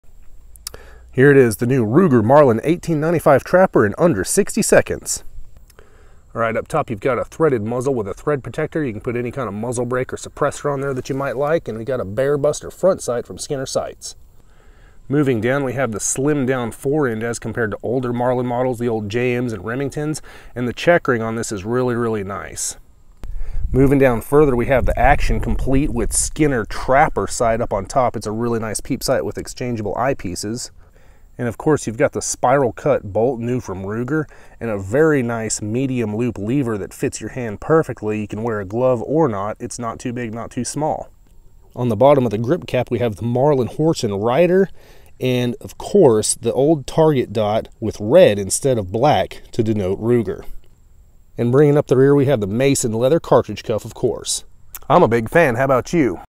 Ruger Marlin 1895 Trapper!
Quick look at the Ruger made Marlin 1895 Trapper 45-70!